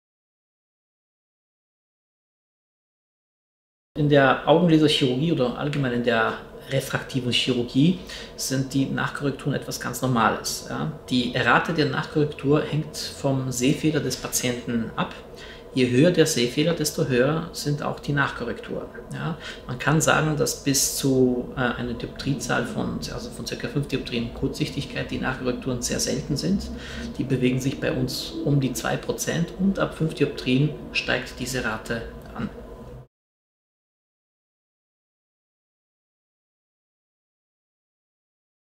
Die Wahrscheinlichkeit einer Augenlasern Nachkorrektur
Keine Angst: Nachkorrekturen sind in der Augenlaserchirurgie etwas ganz Normales. Die Rate hängt vom Sehfehler des Patienten ab.